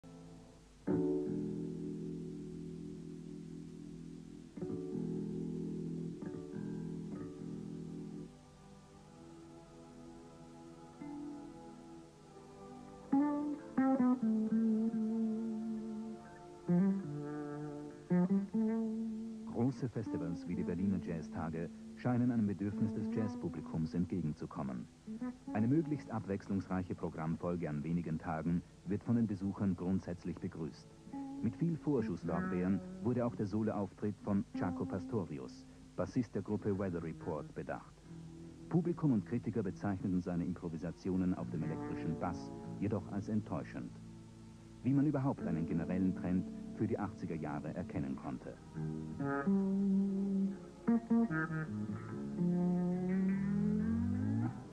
Berlin Jazz Festival 1979 (Jaco at soundcheck)
Berlin Jazztage
Berlin, Germany
Credits: Francky'85